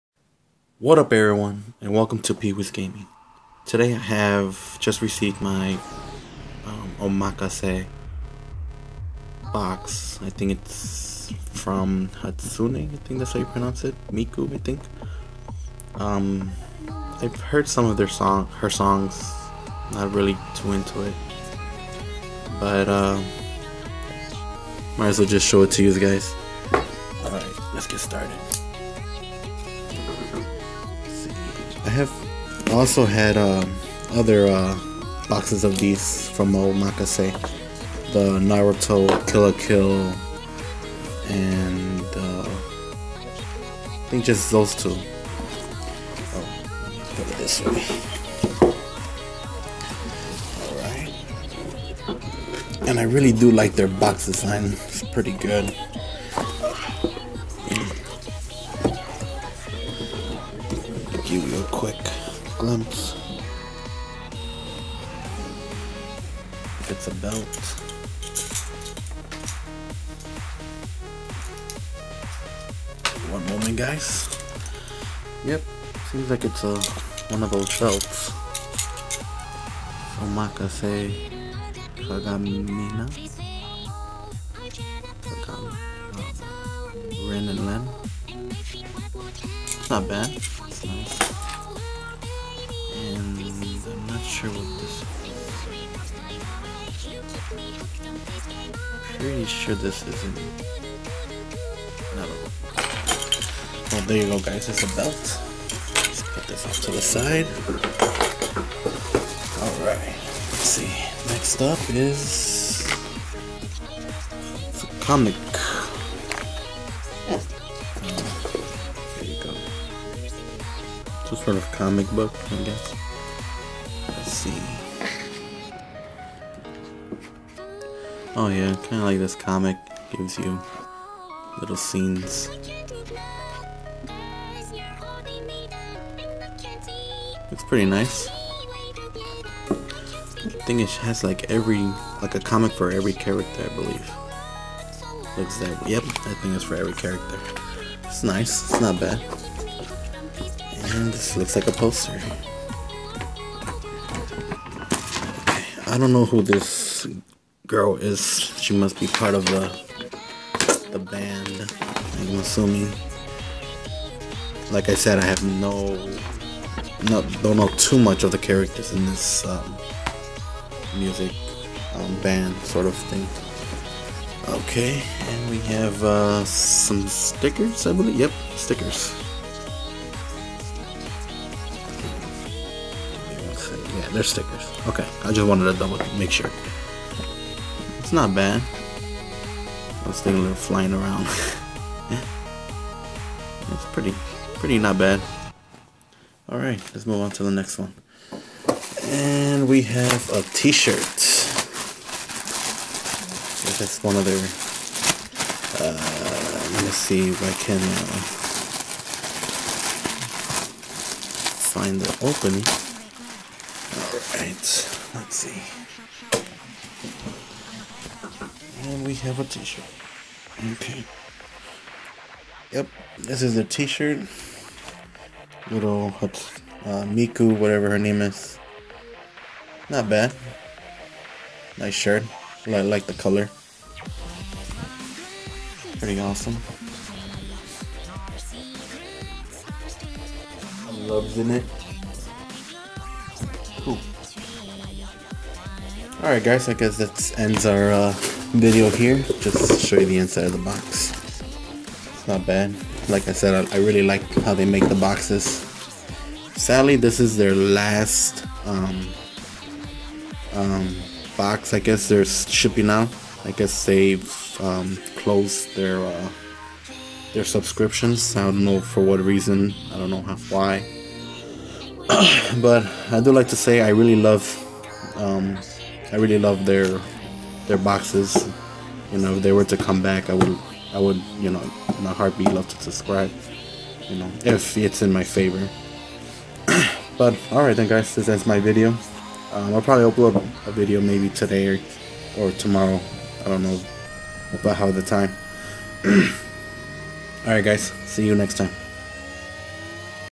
Unboxing Miku Hatsune (Omakase Farewell) v.v
Hey everyone hope u like the video and music and i am into it but ive never look into it that deep to know a lot and i hope u like the  gif i added on there never done it and am really glad i got it down LIKE COMMENT SUBSCRIBE
I also i to say farewell to omakase and thanks for all the goodies they have offers to us